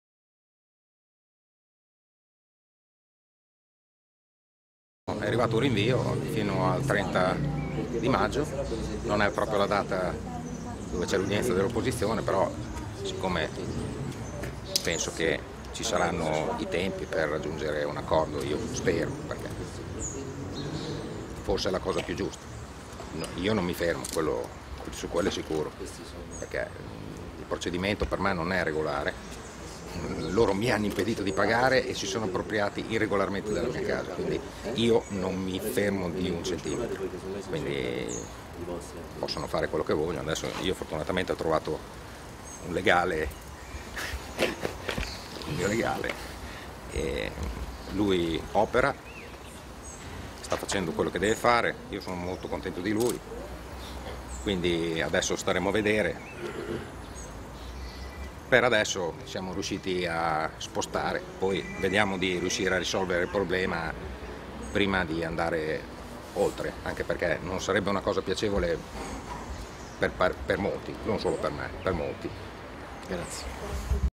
Manca un medico legale, rimandato lo sfratto
Ravenna, Stefano Tassinari dovrà lasciare la casa con la sua famiglia il 30 maggio